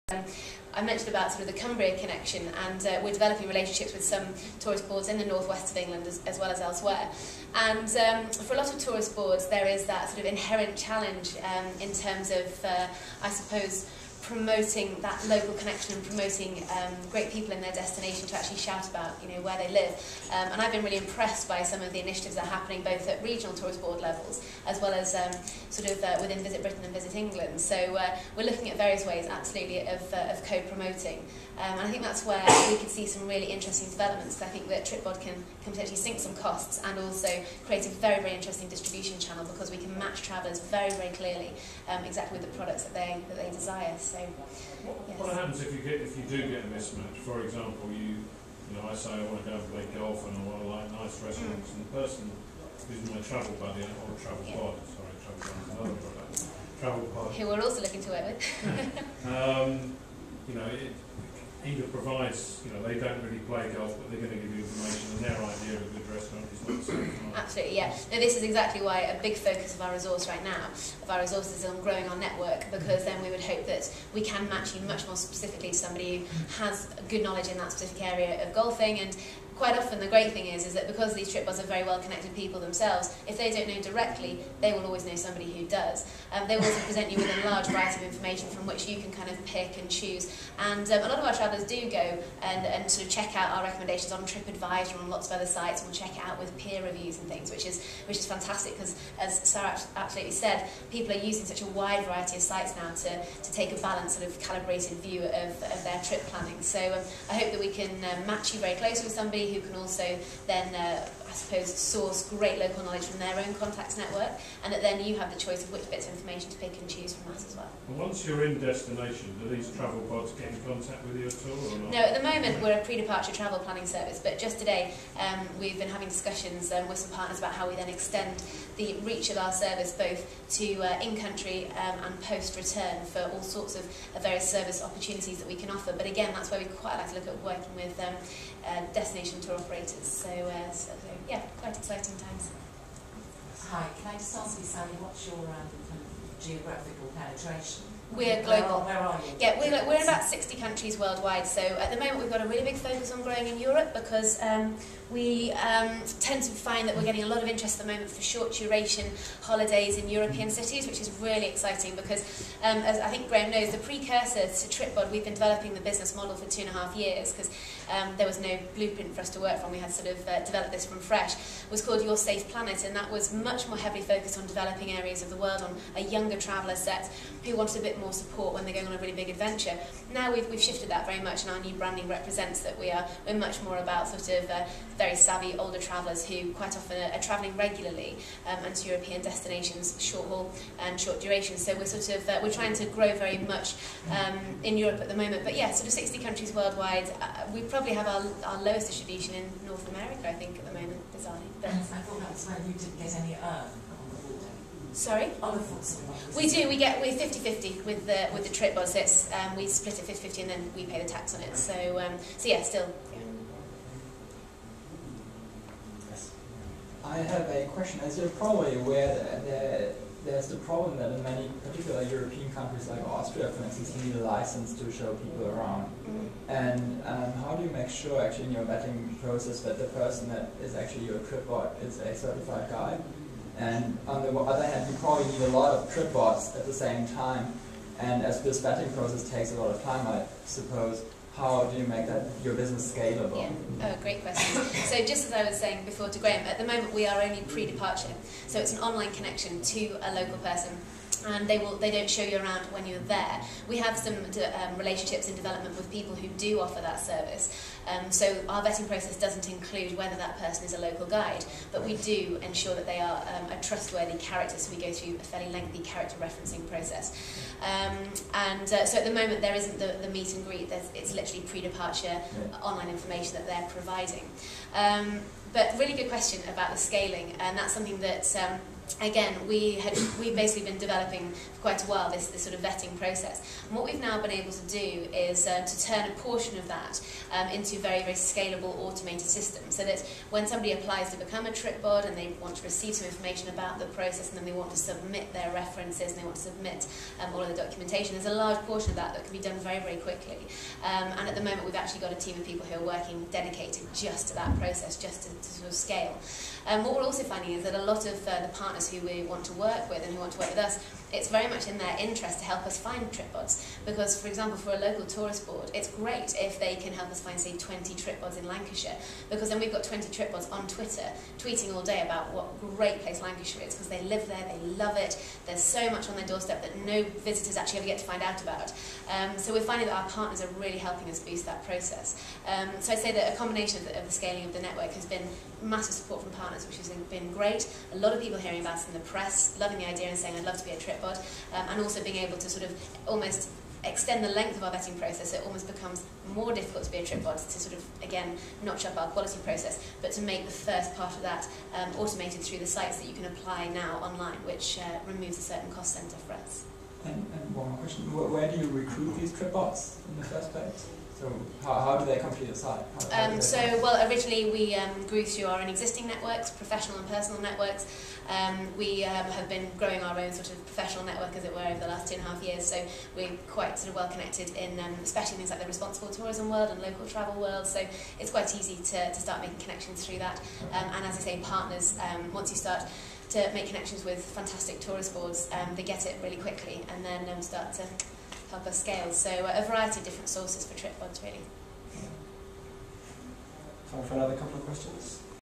ETOA Sally Broom Tripbod QA
Sally, CEO and founder of Tripbod, answers questions at ETOA conference (European Tour Operators Association).
Sally talks about plans for the future of Tripbod including scaling our network, answering the traveller needs, extending the service and where the business is.
Sally answers the questions 'how do you scale whilst keeping quality assurance, and how does certification work at Tripbod?'